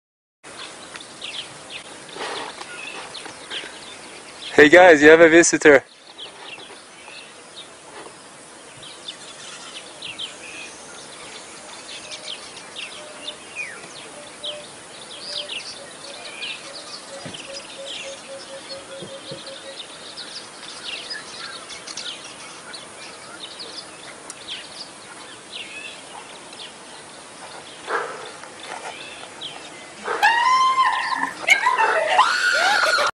massive lizard scares tourists after sneaking into tent 720p
Lors d'un safari en Afrique du Sud, un groupe de touristes a été surpris par la présence d'un énorme lézard. Des images de «Davidkrug» et «Krager Sightings».